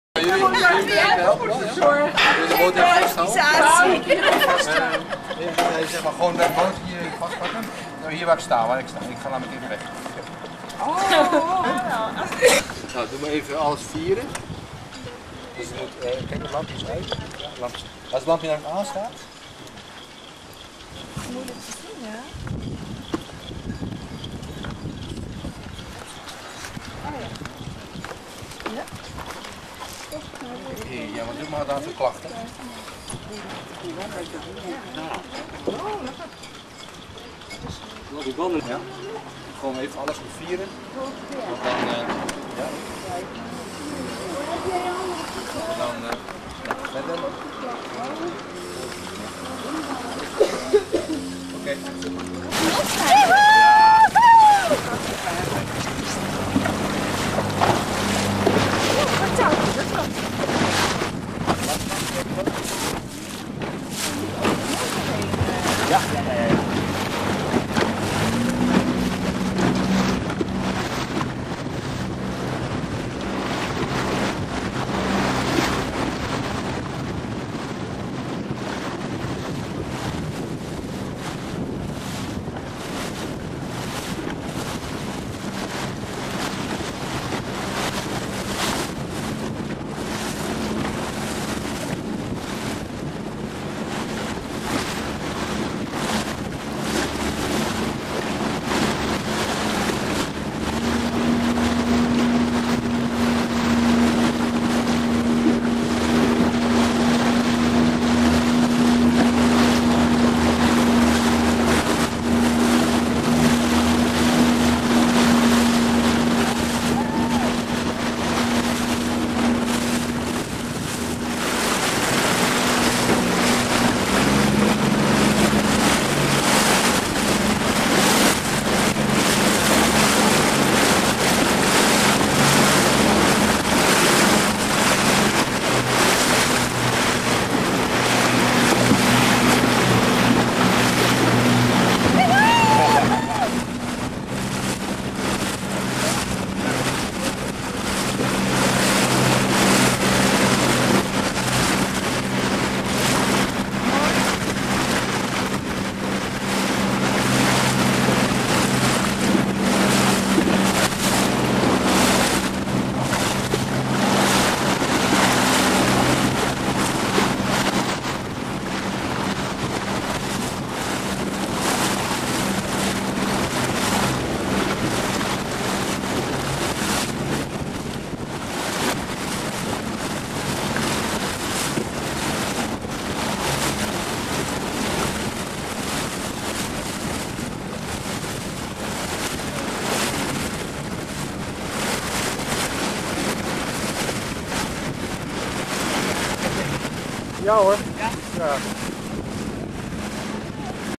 sjors zeilen.wmv
Zeilen op de Lossdrechtse plassen in een aangepaste boot voor gehandicapten